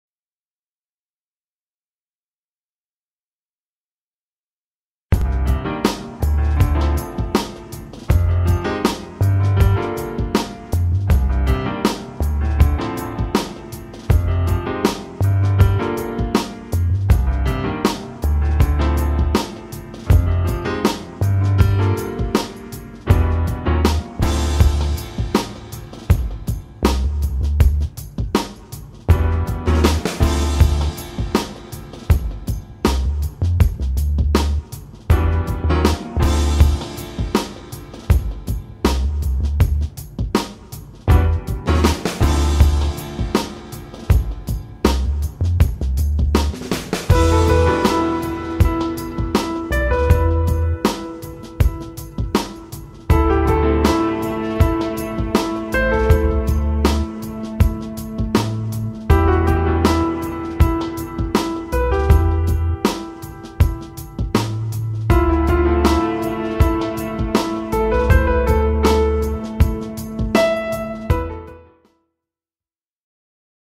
Volkswagen passat cc occasion visible à Beaupuy présentée par Car privileges
Cette Volkswagen passat cc occasion disponible à Beaupuy dans le 31 est mise en vente par Car privileges - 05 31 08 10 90 - tous les détails sont